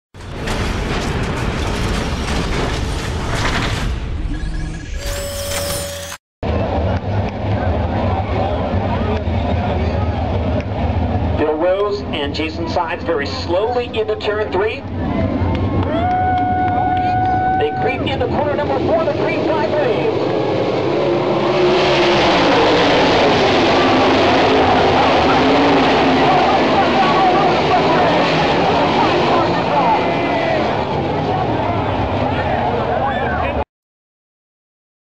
Steve Kinser & Kerry Madsen's violent wreck
Steve Kinser and Kerry Madsen got together on the opening lap at the I-96 Speedway on May 30th, 2011 for the World Of Outlaws Rumble In Michigan. Louie Carufel Was also involved in the wreck as his 47x came barrel rolling down the front stretch. All drivers got out of their race cars and walked away under their own power.